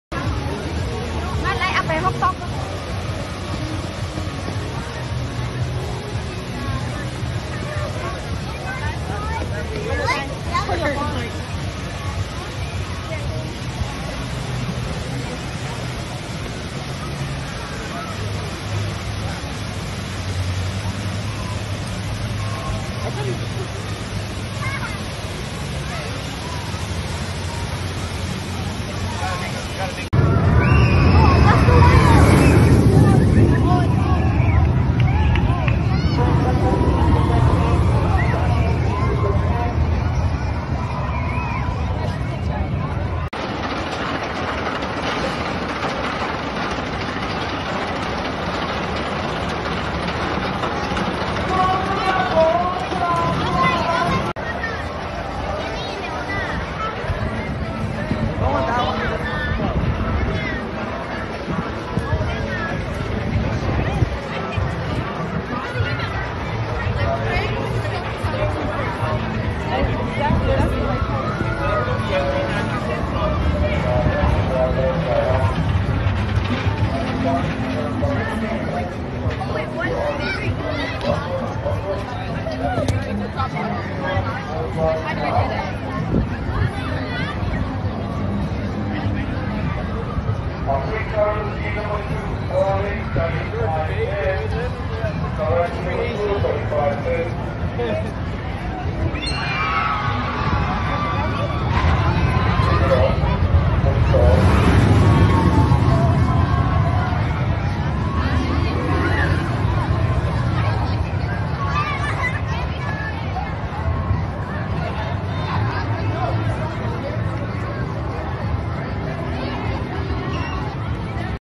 Canada's Wonderland 2023
So much fun in Visiting Canada's Wonderland with my family.